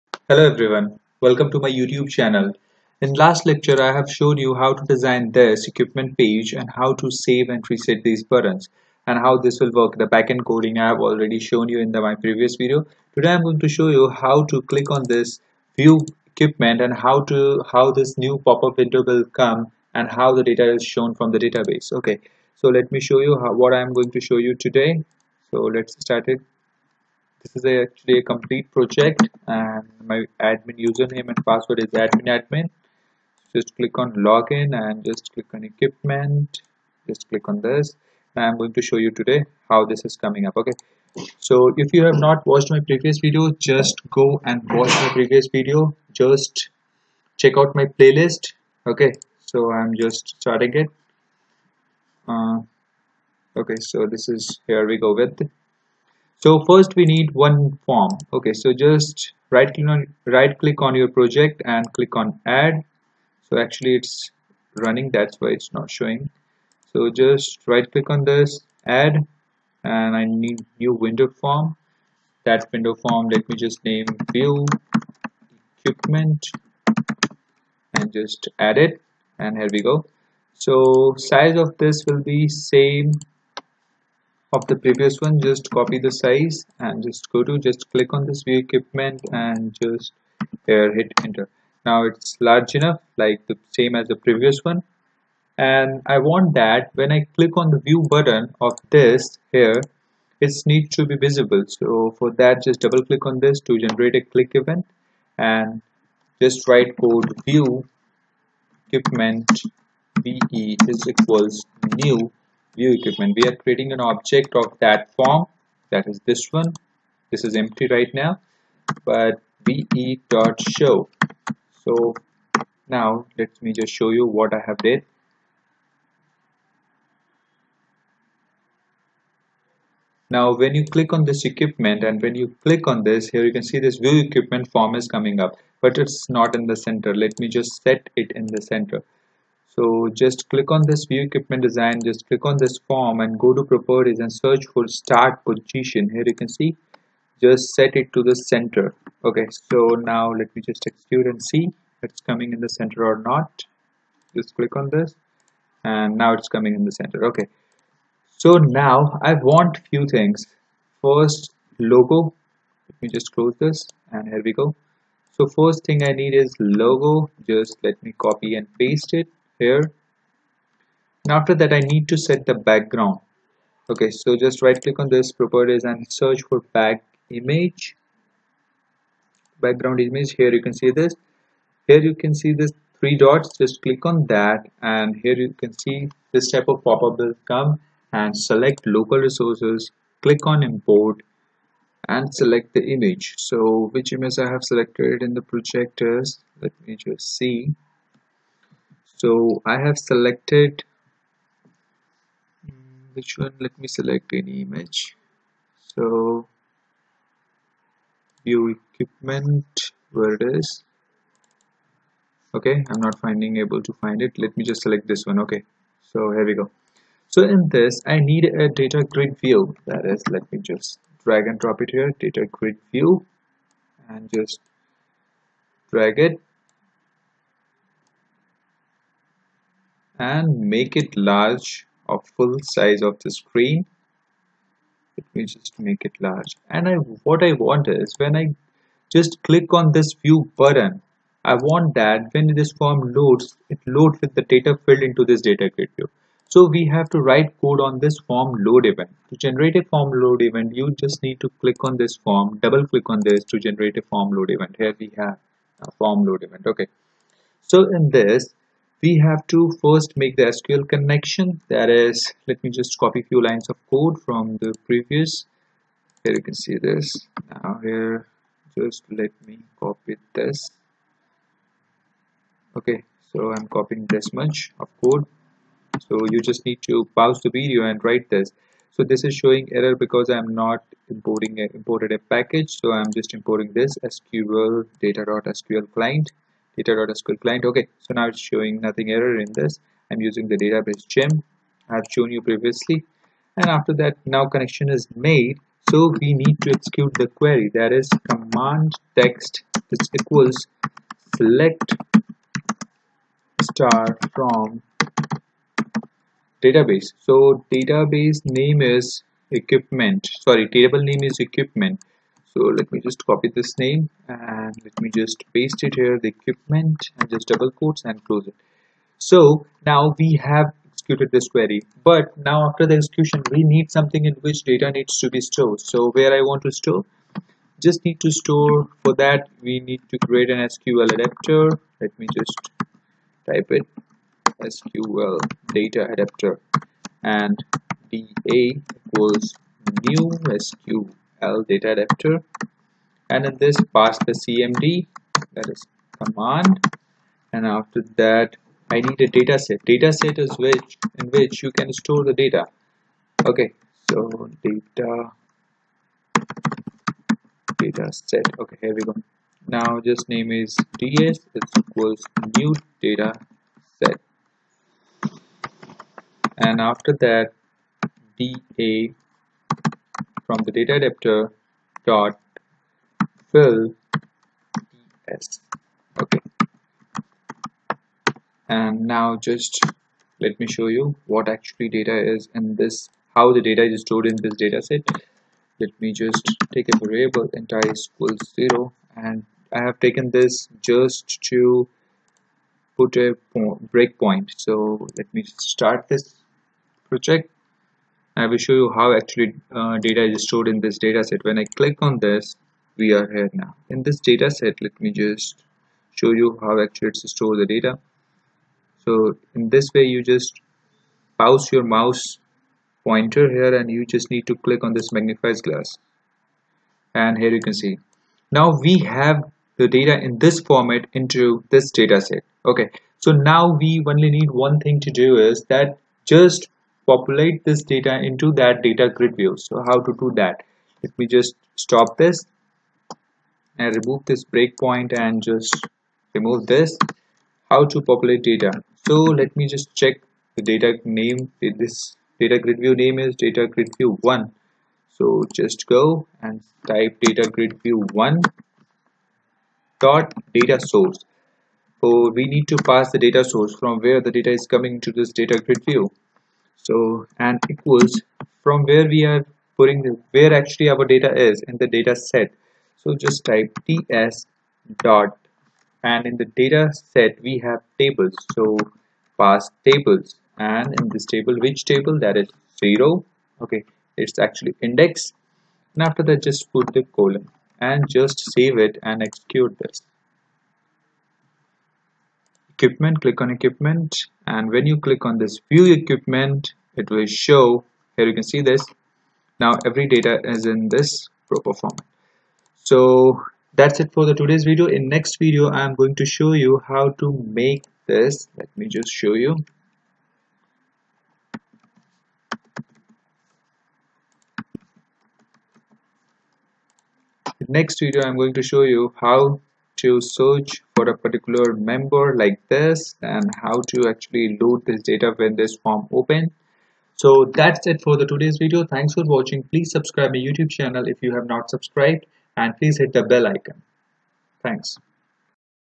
7. Gym Management System - View Equipment Design and Coding
zip Password : btechdays
zip Password : btechdays